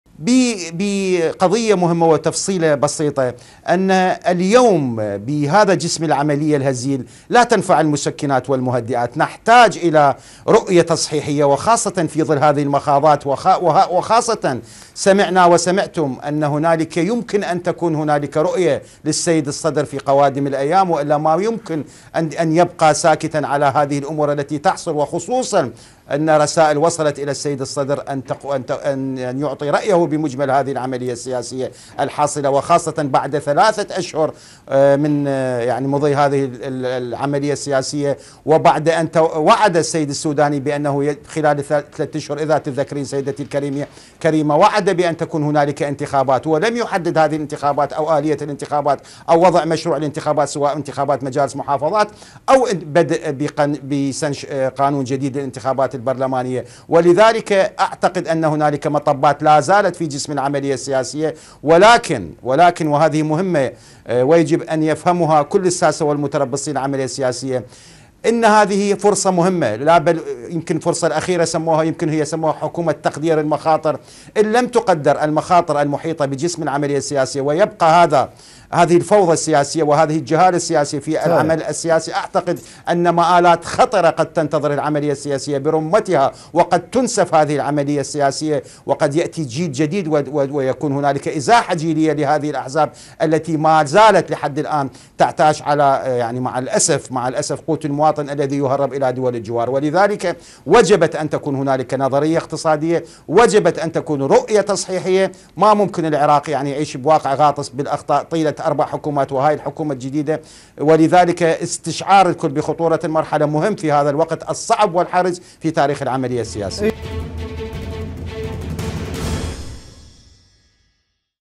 العزاوي: هذا ما سيفعله مقتدى الصدر الايام القادمة
موقع قناة التغيير الفضائية  الالكتروني